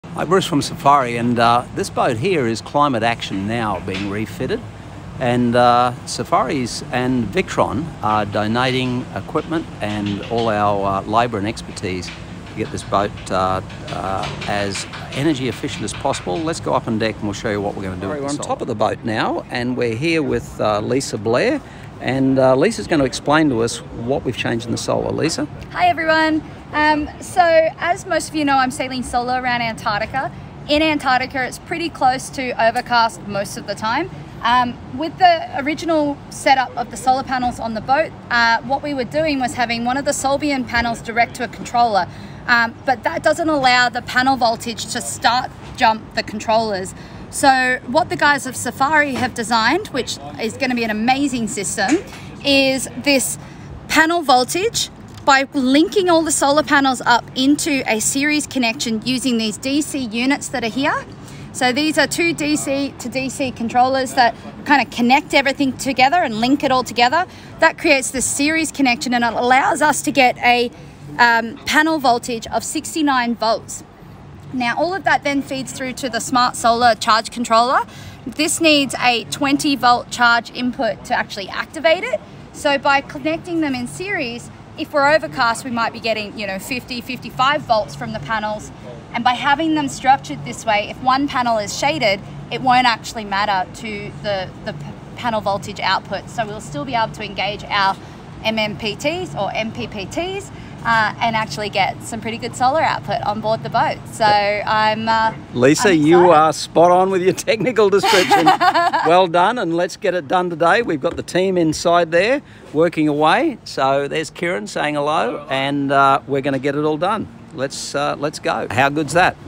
Safiery & Victron Energy Team up with Lisa Blair for Smart Solar Design circumnavigating Antartica
Solar irradiation in Antartica is poor. Lisa Blair’s “Climate Action Now” will need every drop of solar power we can muster. Lisa is sailing around Antarctic aiming for a world record. The B&G Auto-steer consumes a lot of power in heavy weather as does the satellite transmission equipment. It’s fitting that Victron Energy, global leader in marine power electronics is a sponsor. Solbian, the leader in marine flexible solar panels will have over 1,000W of solar from 13 panels of three different size and specifications. Getting the maximum efficiency from the solar is the hard part and that’s where Safiery come in. The dilemma is joining the same side panels in parallel results in some panels with poor output because of keel angle and they are a very low PV os 20V. This also means a short period of solar production. Linking panels in series means shading effects and differing solar panel specs would choke power output. However, Safiery have invested in their own DC to DC for Solar that is Victron MPPT compatible. This results in 3 strings of solar with a PV of around 69V. The panels are selected for best results 1) Keel to Port 2) Keel to Starboard 3) Either or flat sailing. Each string goes to a separate victronenergy Smart Solar controller that takes up to 100V PV. The results will be shared live from Lisa’s Website so other sailors can see this innovation in action! @lisablairsailstheworld